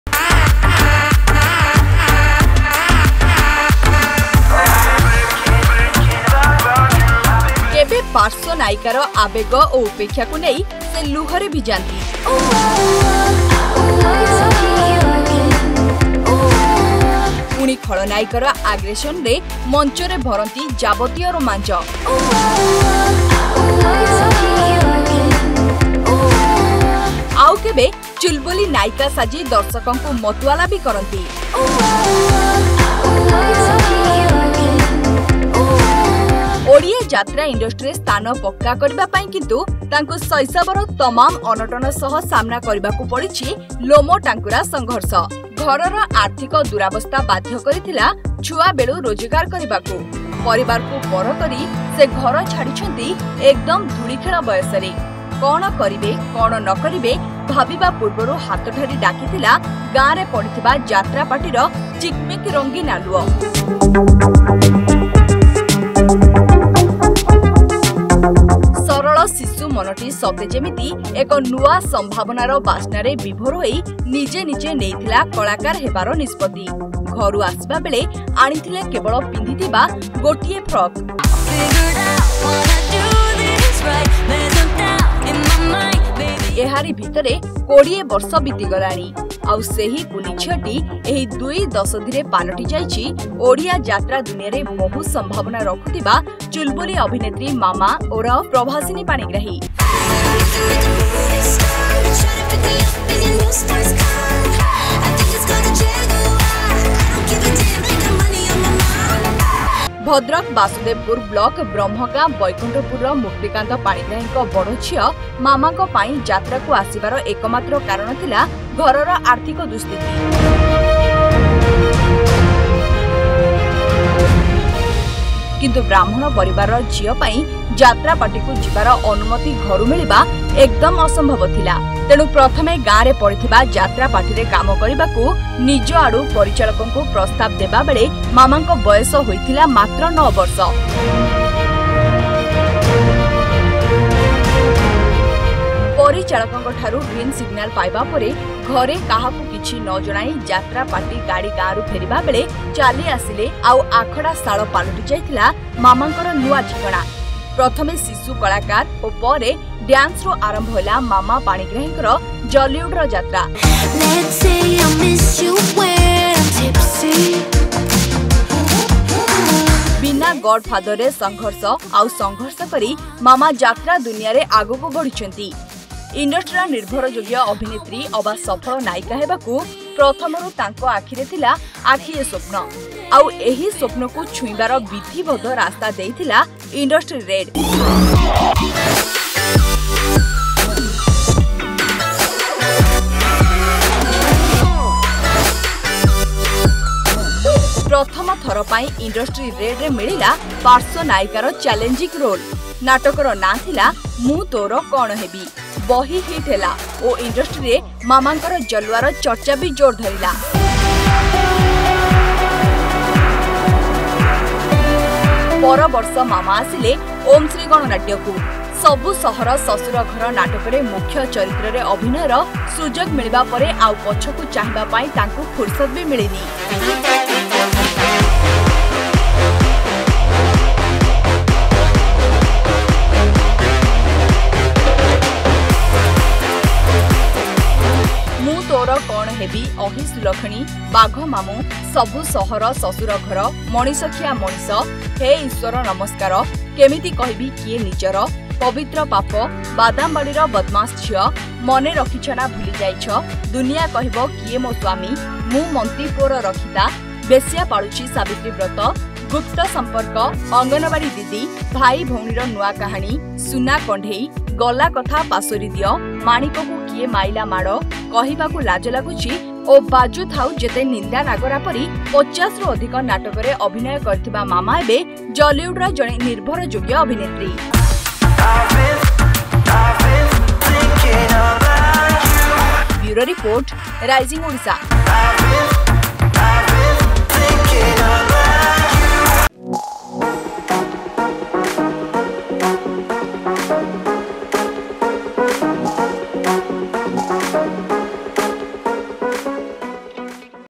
Story of Jollywood Actress Mama | Prabhasini Panigrahi | ଓଡ଼ିଆ ଯାତ୍ରା ଦୁନିଆର ଚୁଲବୁଲି ଅଭିନେତ୍ରୀ ମାମା
This is the story of Odia Jatra actress Prabhasini Panigrahi or popularly known as Mama is the eldest daughter of Muktikant Panigrahi of Brahmagaon Baikunthpur, Basudebpur block, Bhadrak.